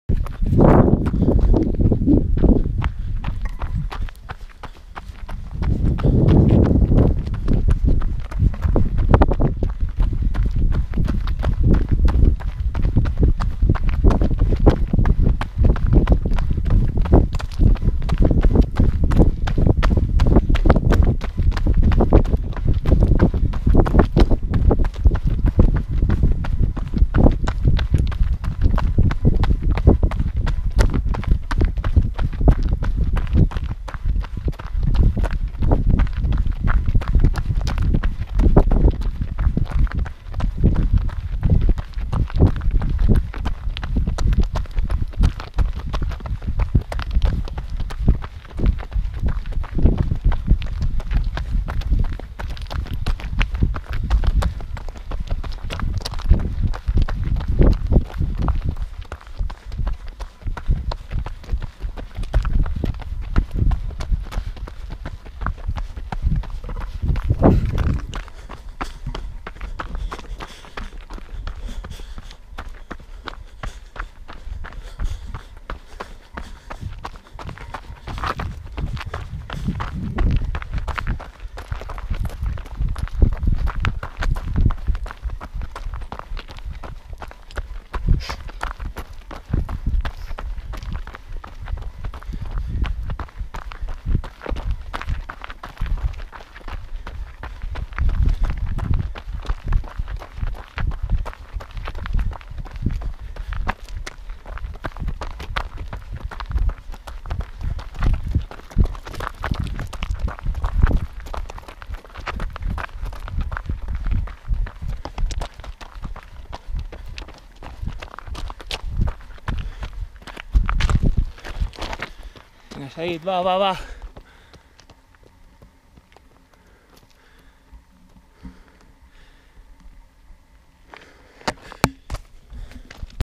Zaid ait malek Pas del Bou, km 52 Ultra Pirineu 2018
2º clasificado en meta tras Jessed Hernandez.
Parque natural Cadi moixero